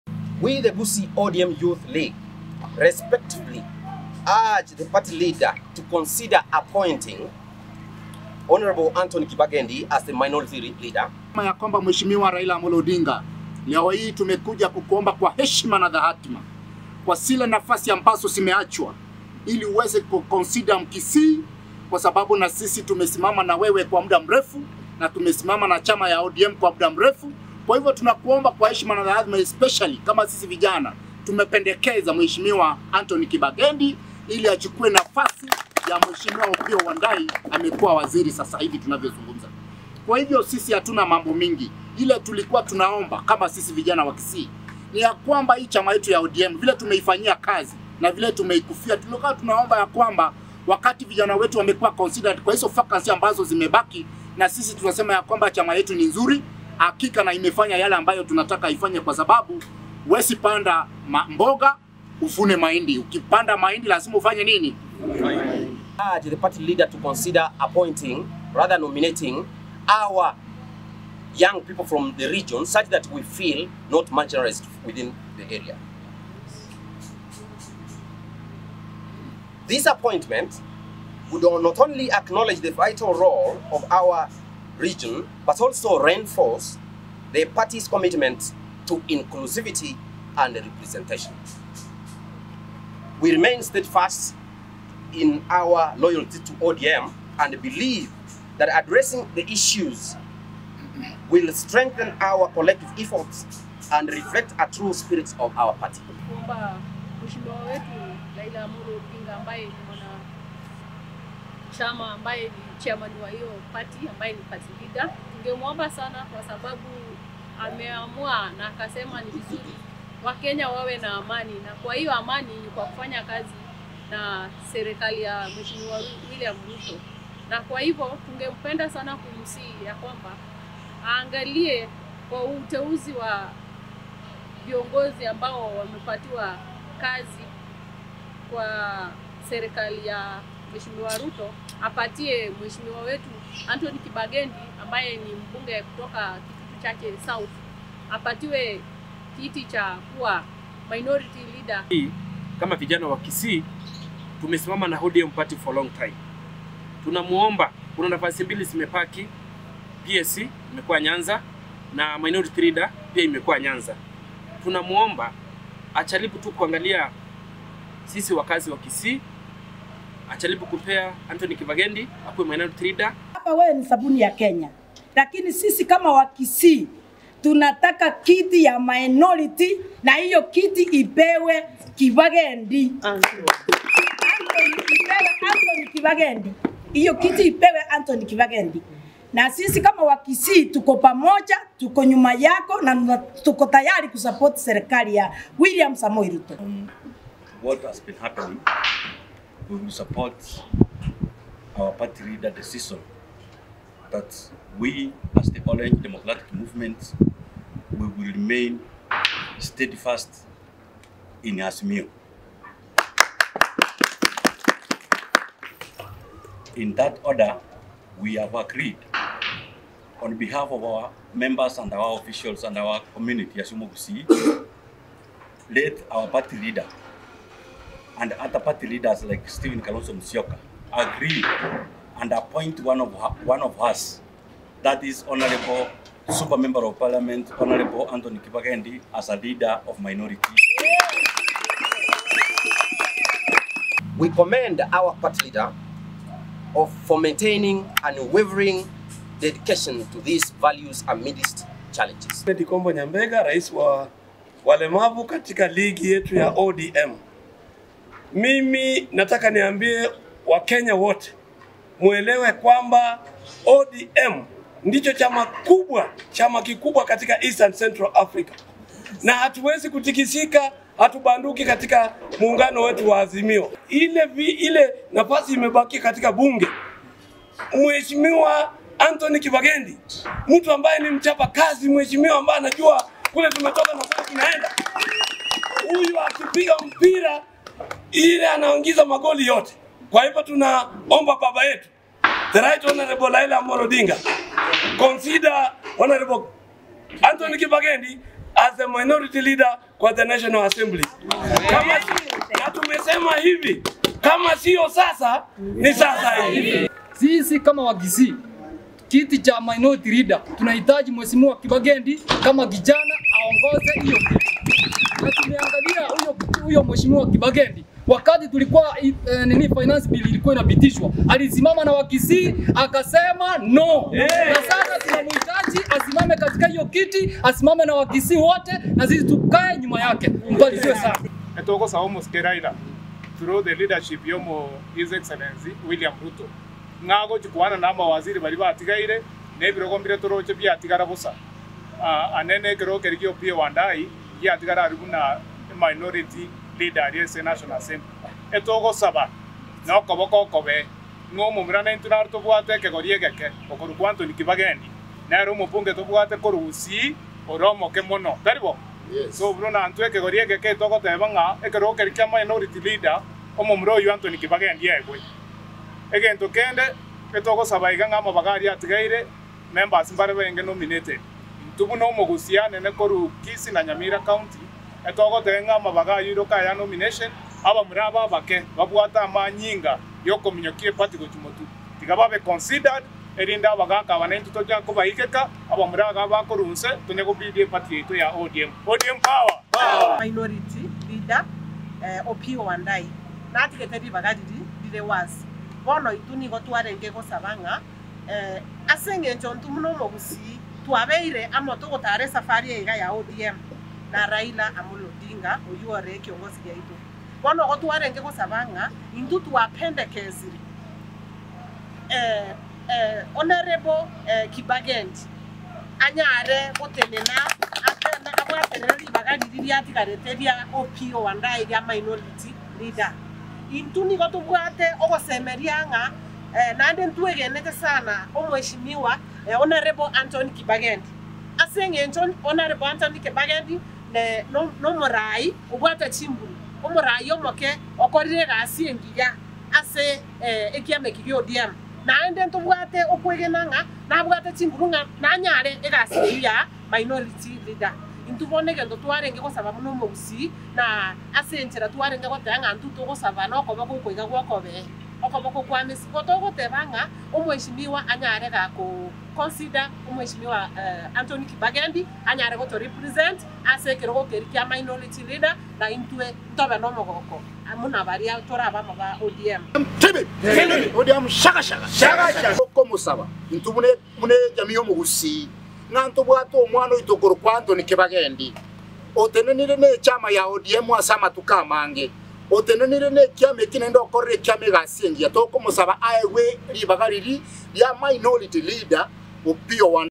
Gusii ODM Youth League Endorse MP Anthony Kibagendi for Minority Leader
MESSENGER 045 NEWS TV is the upcoming leading Online Tv in Gusii  Region and East Africa Basically for News.